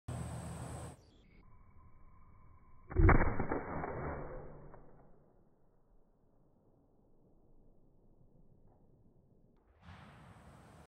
Kammerlader test shot, in SloMo
Apprentice Andrew, getting some Kammerlader trigger time. Outstanding, eh?